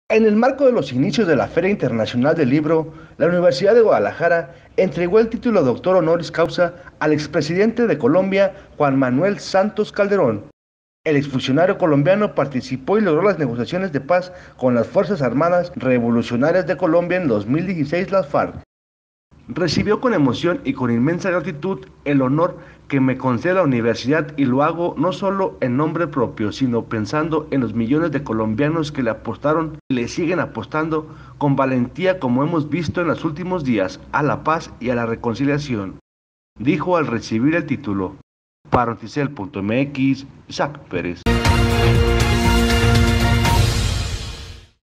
Honoris Causa a Juan Manuel Santos
Otorgan el título al ex presidente de Colombia por la trascendencia en sus aportaciones en materia de negociación, paz y reconciliación en beneficio de la humanidad